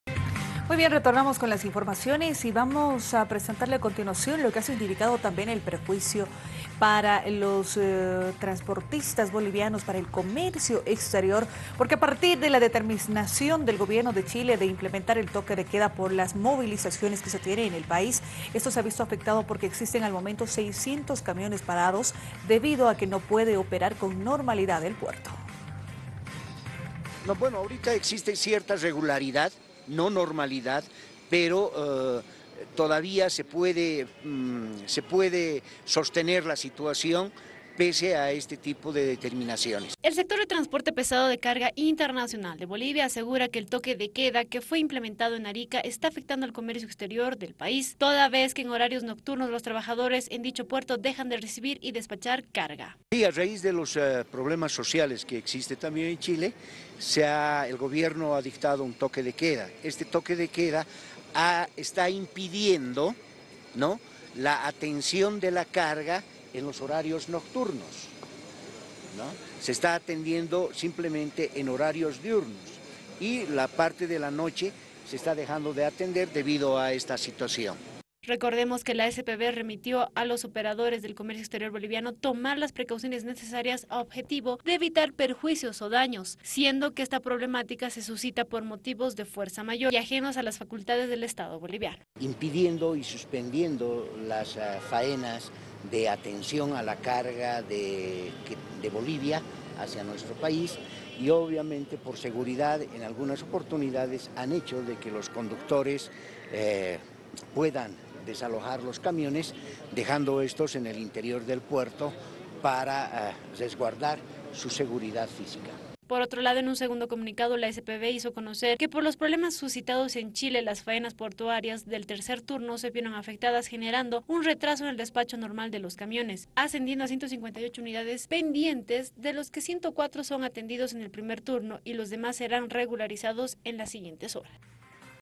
Chile: El comercio exterior boliviano se ve afectado por el toque de queda
El toque de queda en Arica - Chile afecta una vez más al comercio exterior boliviano al paralizar el trabajo de faena en horario nocturno. Cerca de 600 camiones se ven imposibilitados de realizar el cargo y descargo de mercadería.